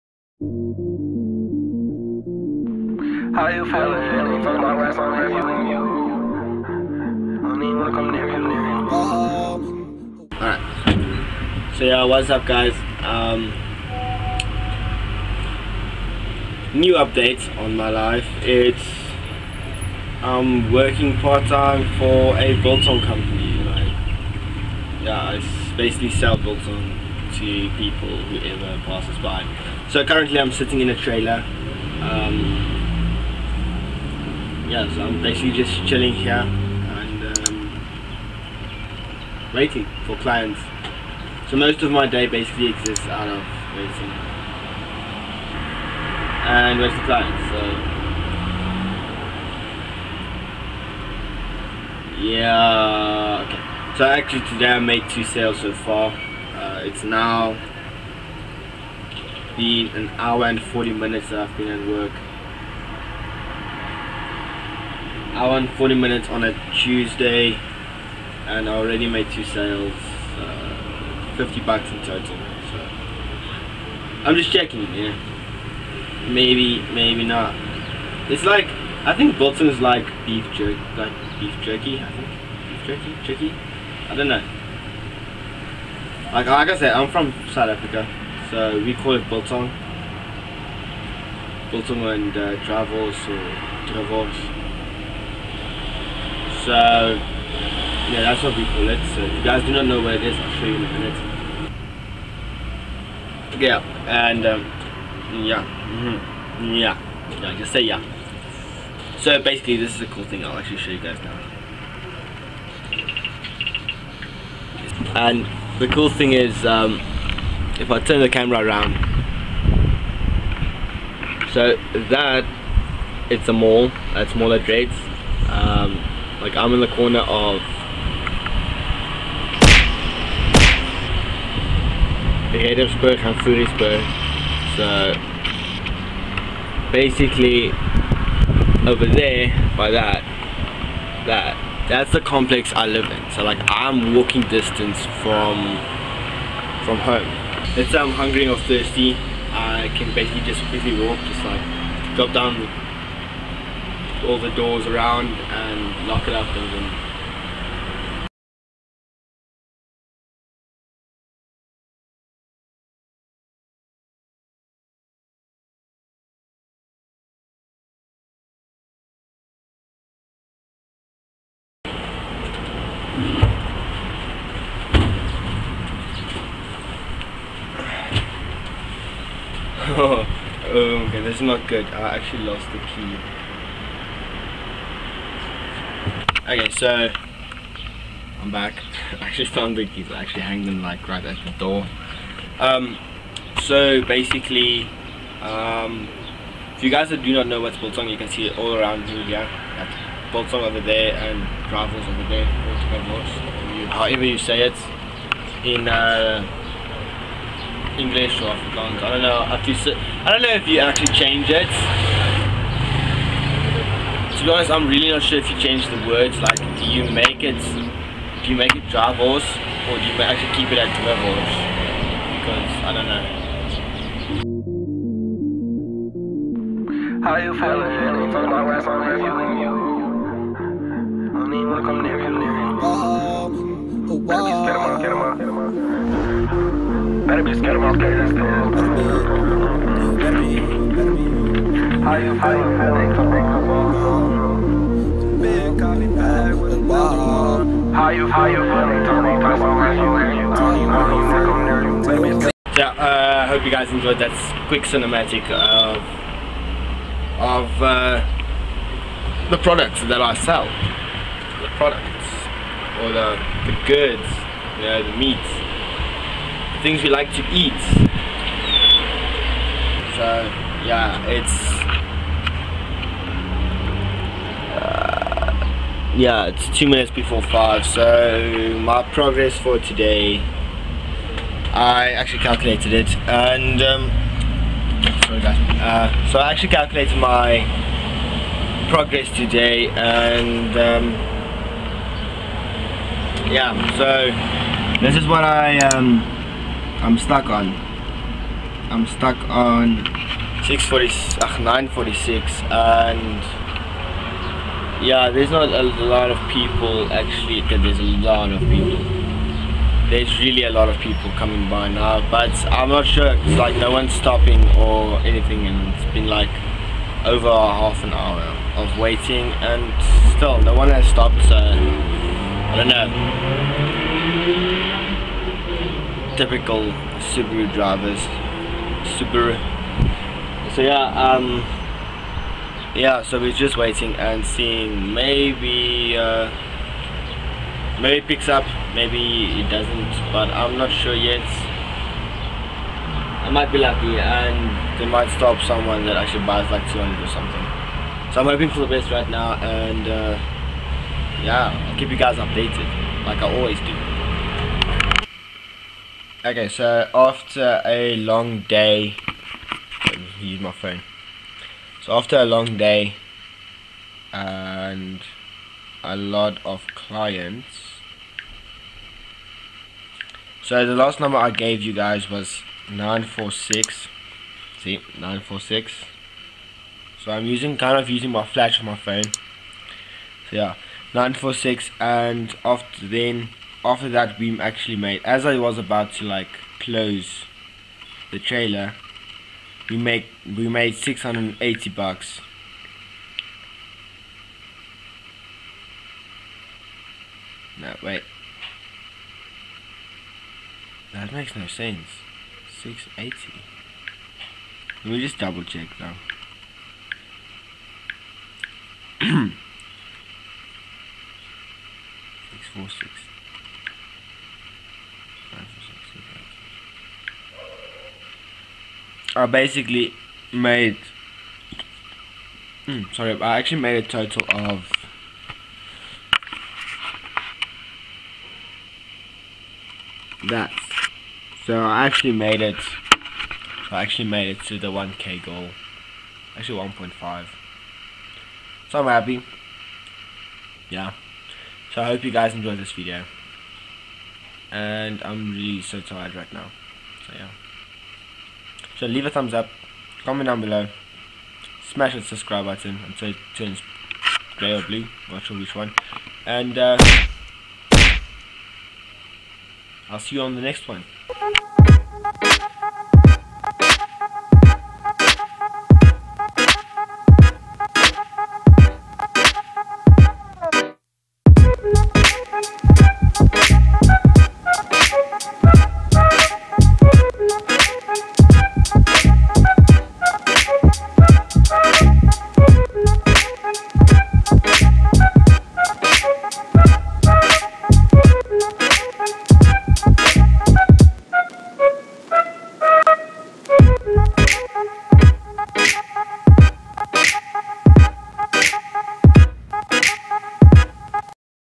Update On My Life Vlog 1 | South African Youtuber
Update on my life from GeelVet Bilton Trailer, Where money is made and biltong to be sold.

Thanks for all the love and support support...!!!